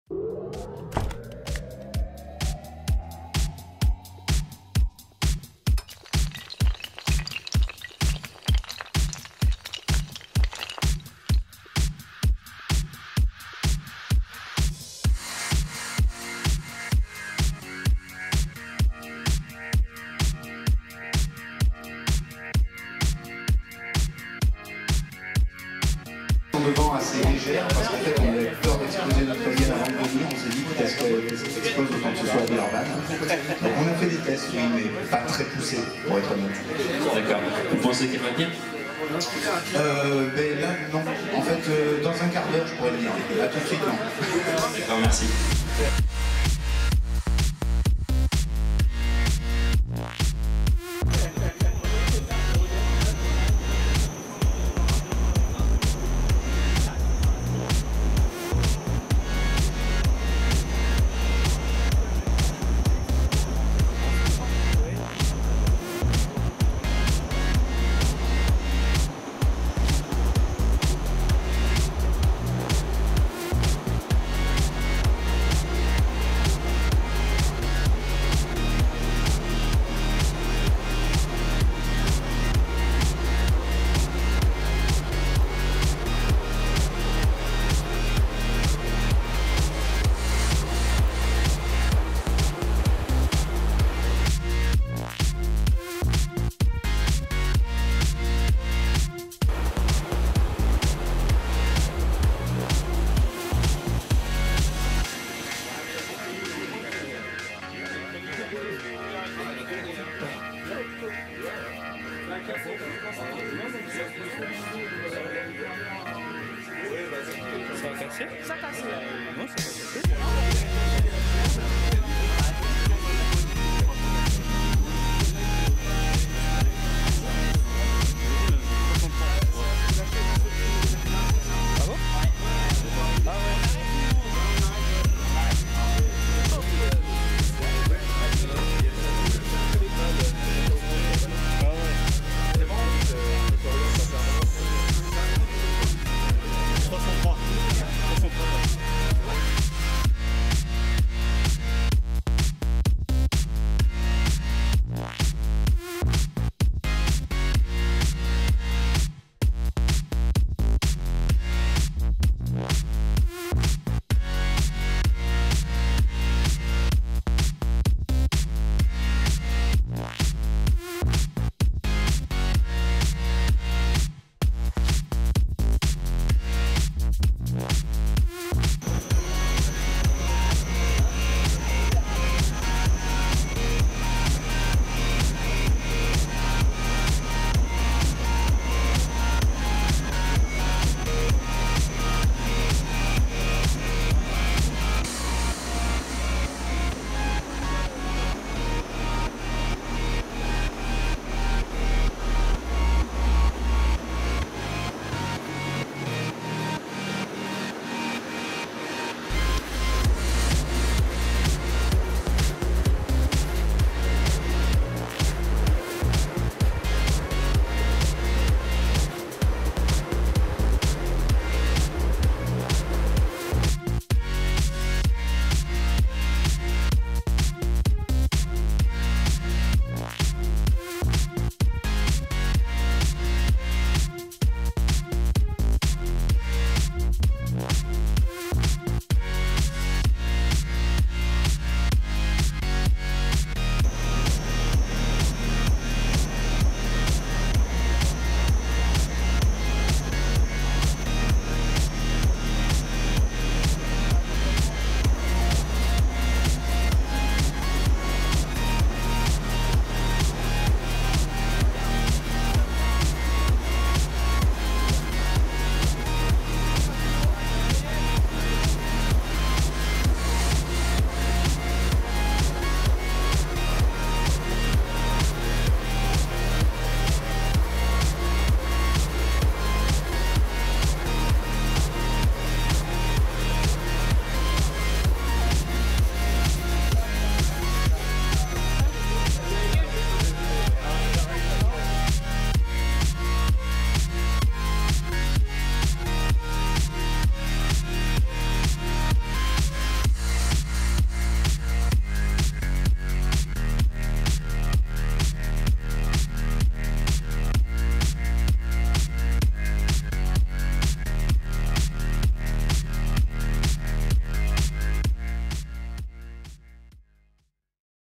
Vidéo des essai en concours GIM'éole 2013 Tulle
Cette vidéo résume l'aventure des étudiants tullistes au concours GIM'éole 2013. Concours qui s'est déroulé à Villeurbanne, IUT Lyon 1. Tous issus de la formation DUT Génie Industriel et Maintenance, chaque année, un groupe d'étudiant se tue à la tâche pour la conception et la réalisation d'une éolienne urbaine.